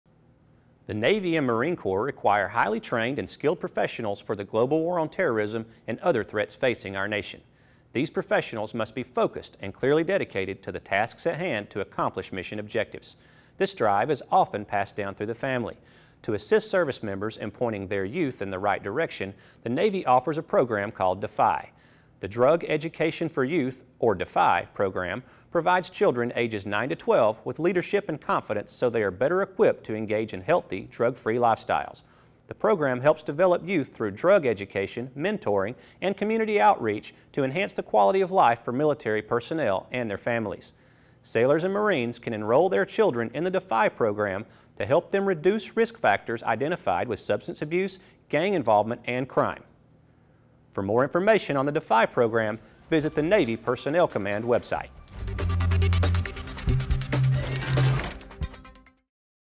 The DEFY Program
Program assist service members in pointing their youth in the right direction.